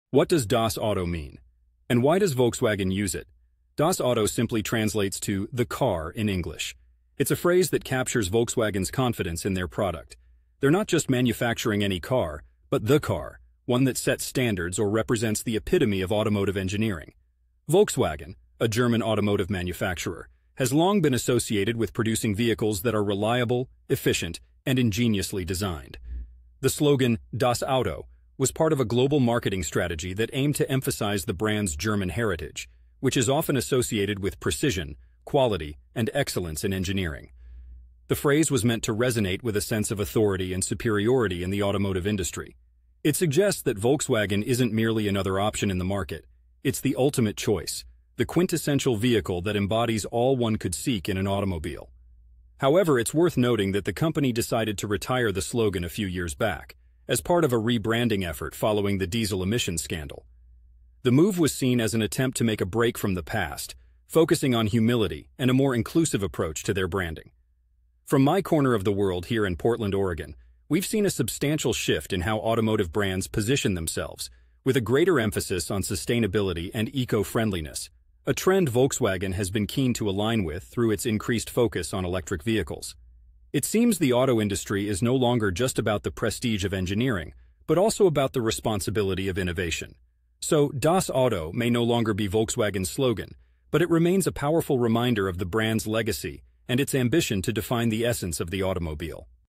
What Does Das Auto Mean And Why Does Volkswagen Use It?
I answered this question on Quora My video answer to the question: What Does Das Auto Mean And Why Does Volkswagen Use It?